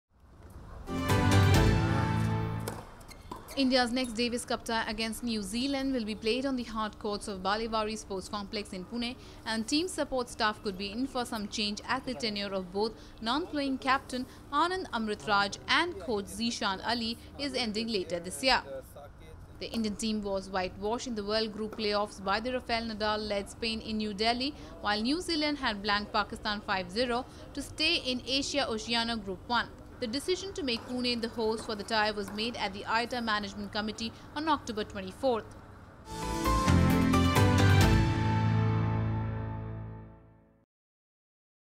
India-New Zealand Davis Cup Tie To Be Played In Pune
India’s next Davis Cup tie against New Zealand will be played on the hard courts of Balewadi Sports Complex in Pune and the team’s support staff could be in for some change as the tenure of both non-playing captain Anand Amritraj and coach Zeeshan Ali is ending later this year.